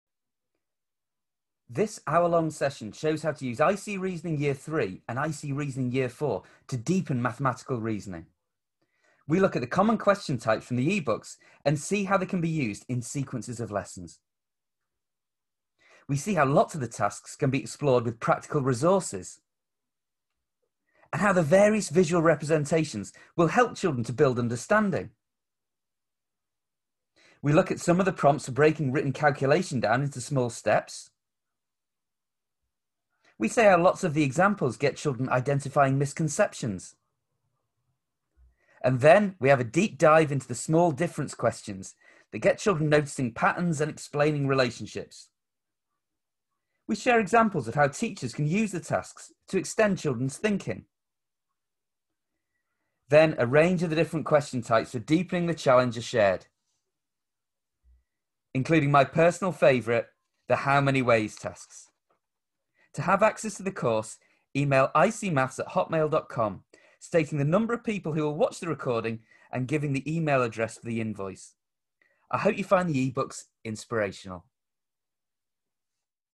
Online Training: Using I See Reasoning Y3 and Y4
Here we have a quick look at the content from the online course about using the I See Reasoning eBooks in Y3 and Y4.